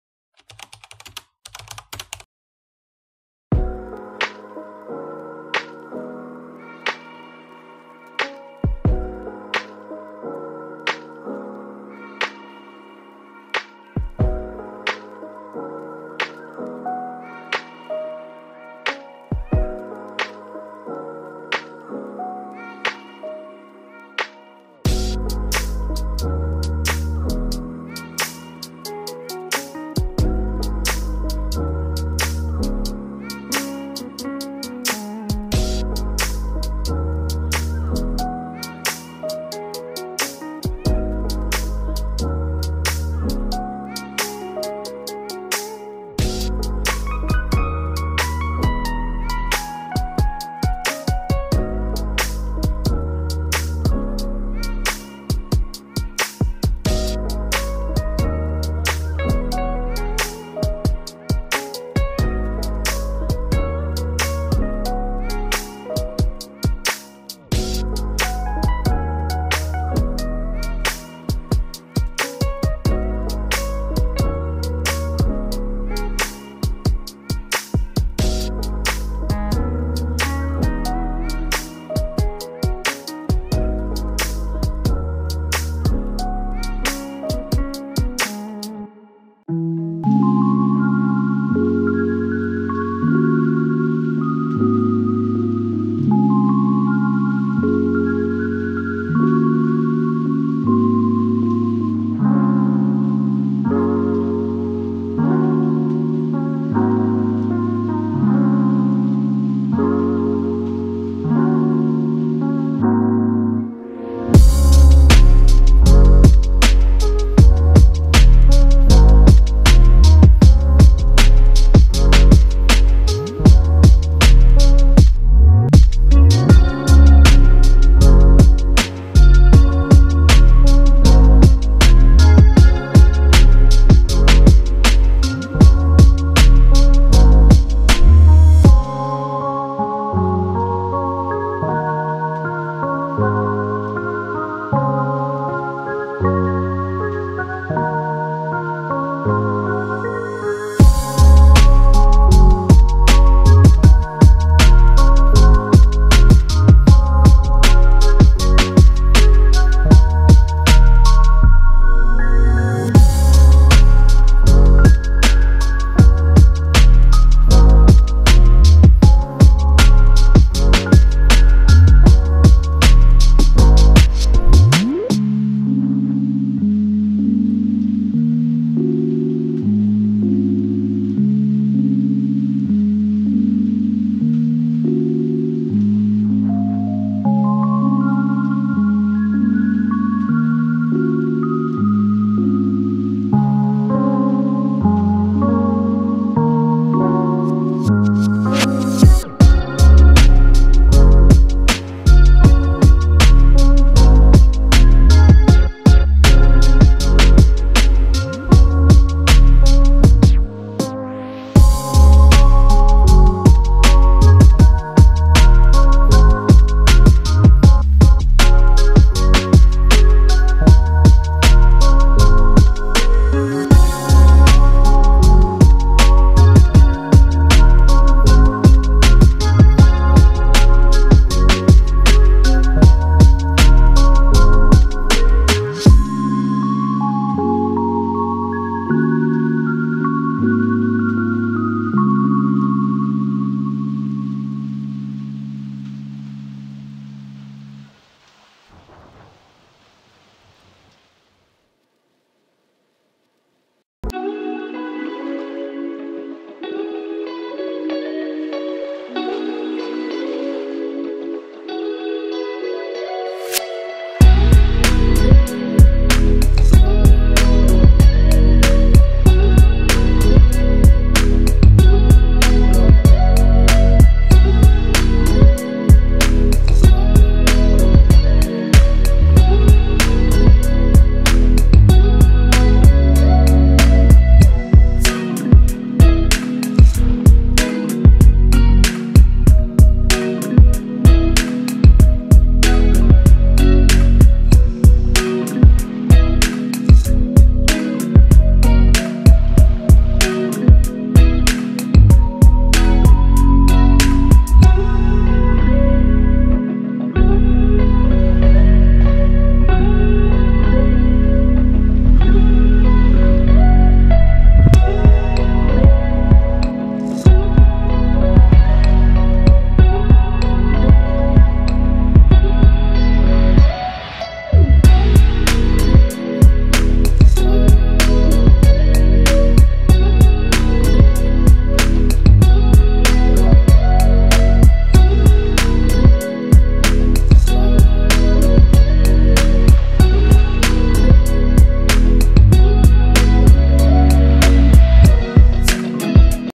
☁️sauna's painting vlog!|opening and painting with Himi Gouache
Hello! Welcome to my first painting vlog :) it's my first one so i don't really know what i'm doing but i hope you enjoy it!